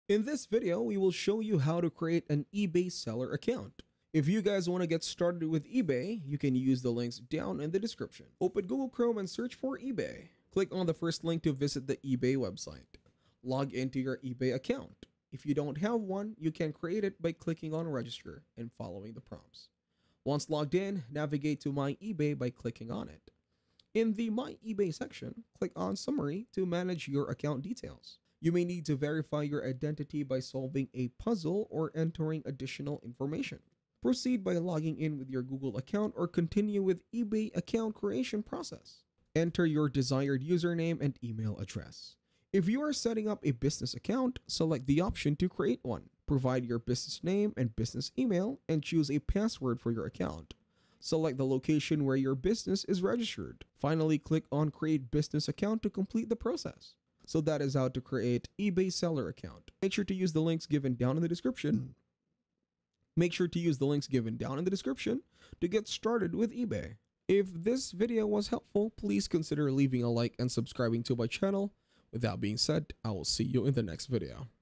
How to Create eBay Seller Account (Easy)
In this video I'll show you How to Create eBay Seller Account